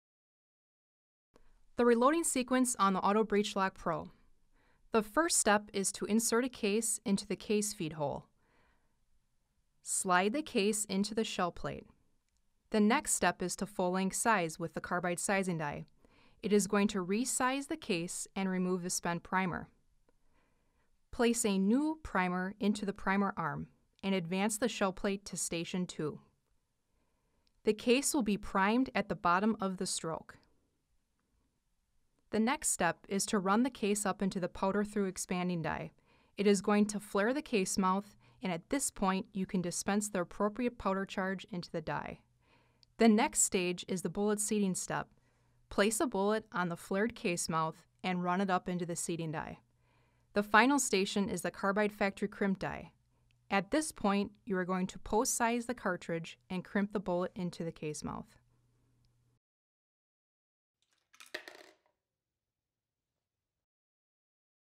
90900 Auto Breech Lock Pro Reloading Sequence
Announcing a new concept in automatic reloading presses. A true progressive press for less than the cost of a premium single stage press. Order a set of Lee dies and a shell plate, and you have everything you need to start reloading.

Shell plate is required to use this press, sold separately.